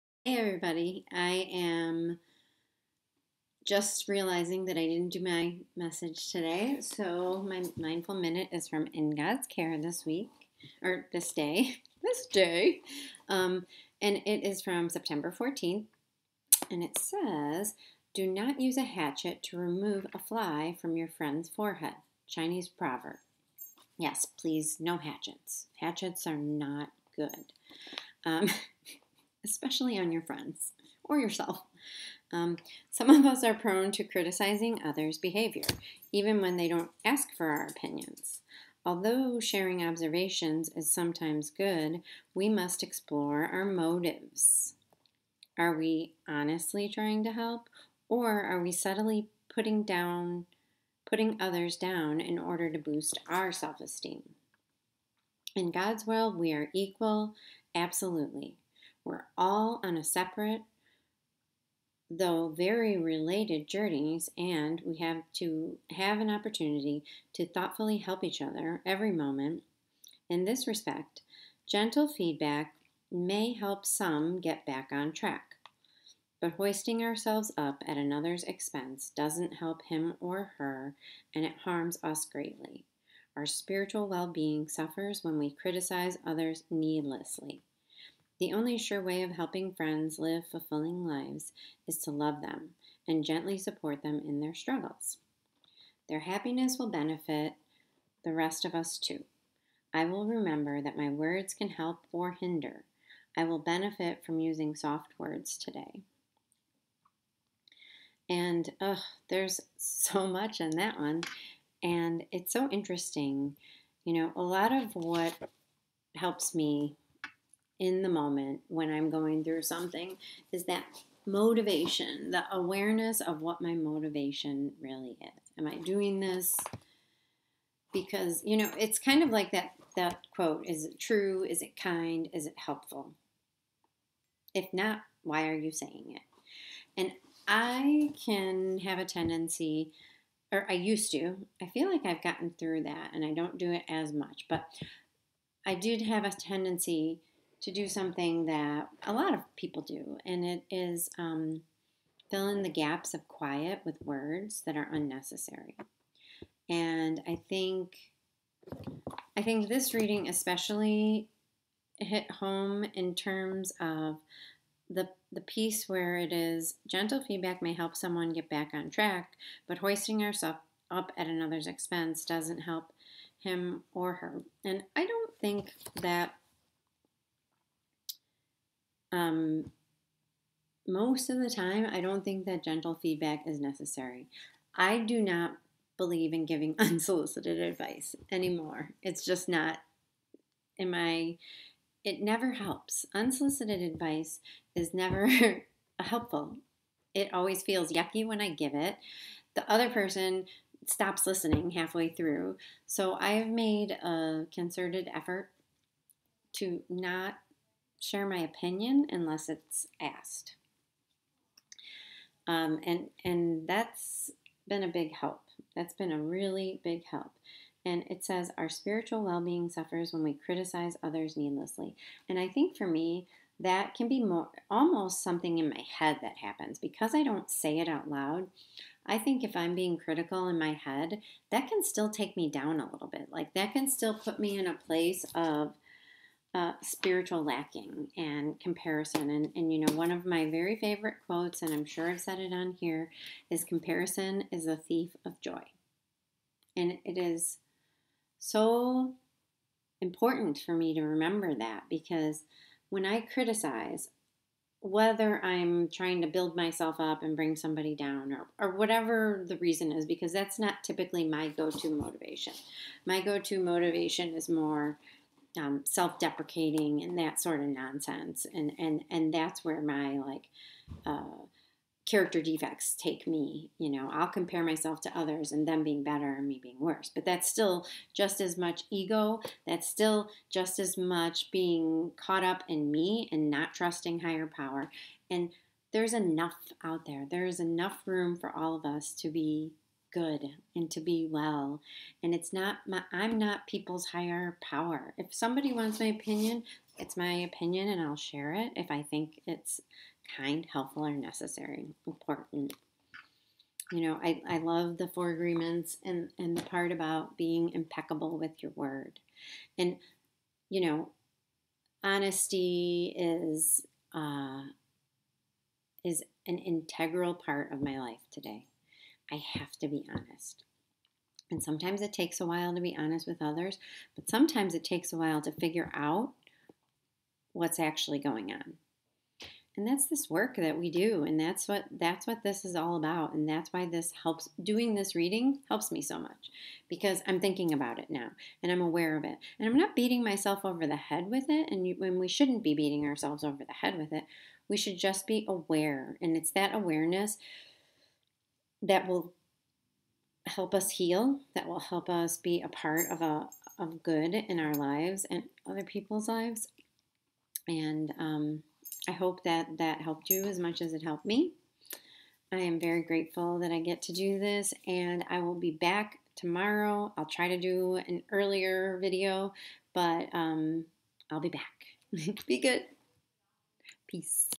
Mindful Minute 9/14/2019
Today's reading is about criticizing others and checking our motivations.  From In God's Care 9/14.